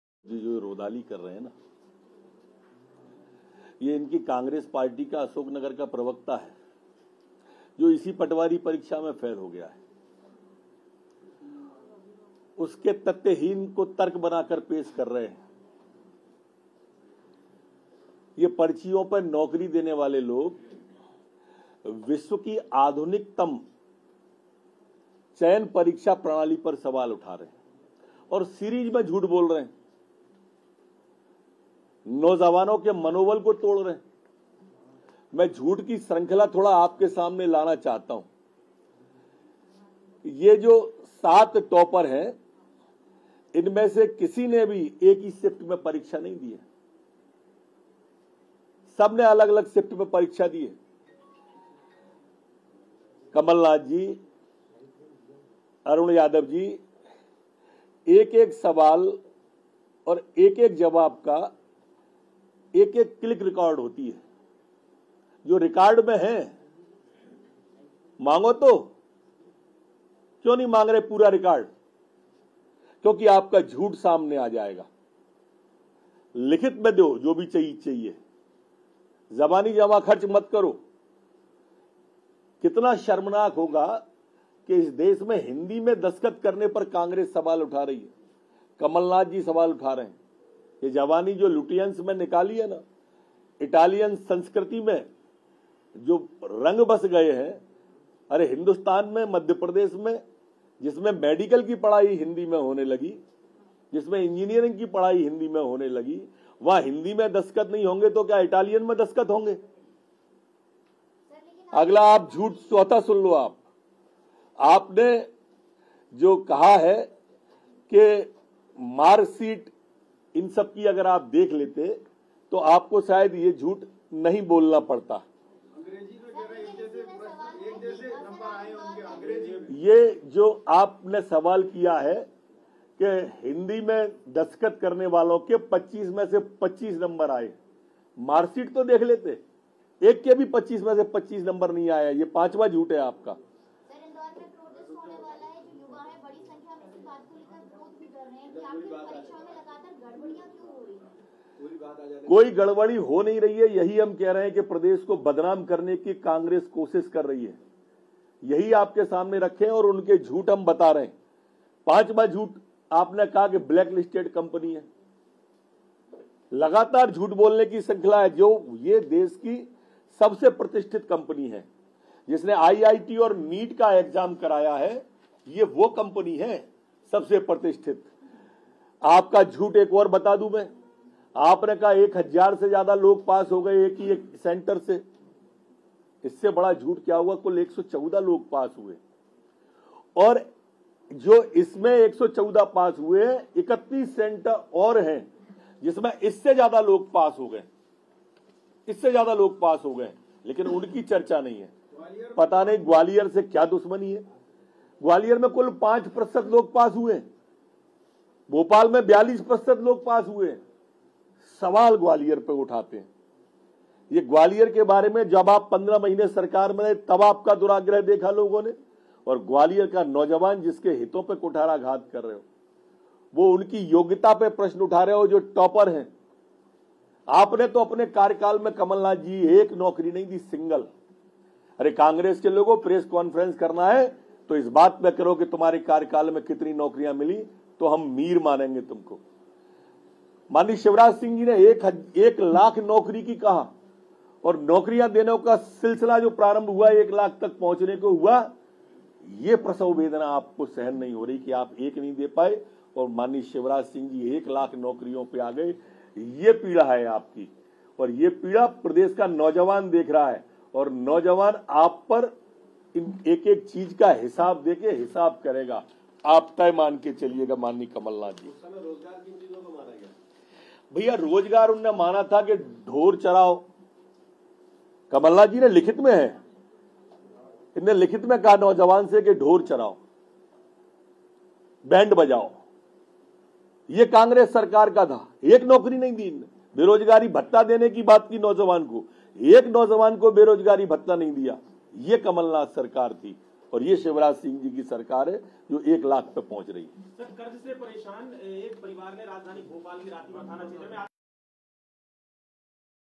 MP Patwari Exam पटवारी परीक्षा में नरोत्तम का बड़ा खुलासा ।
MP Patwari Exam पटवारी परीक्षा में नरोत्तम का बड़ा खुलासा । Hindustan24

 exam,Patwari Result 2023,patwari exam ghotala,mp patwari farjiwada